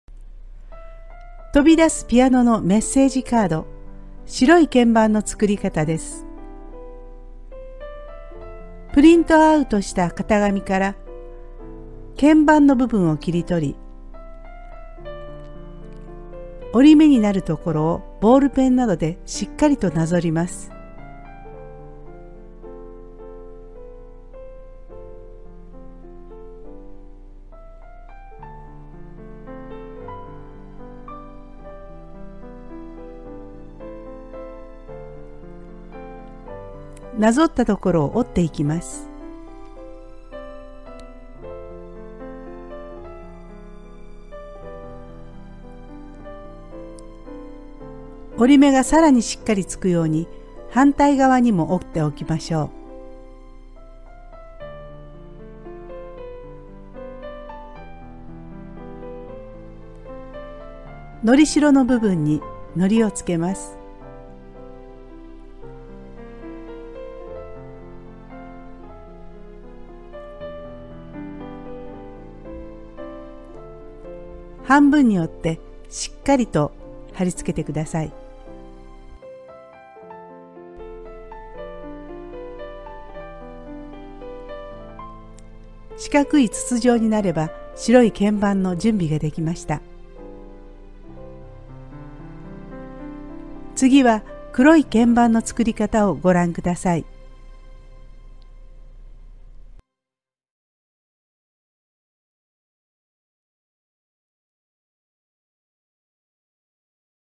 kimie gangi 飛び出すピアノのメッセージカード NEW ①白い鍵盤の作り方
型紙をダウンロードして、作る飛び出すピアノのメッセージカードです。
昨年の物よりより簡単にできるように型紙をバージョンアップしています。
ポップアップカード初心者でも作れるように、作り方も３つに分けて説明しています。
今回は「白い鍵盤の作り方」です。
型紙は、下記のURLからダウンロードしてください。
BGMは即興ピアニスト朝田恵利さんの曲を使わせていただいています。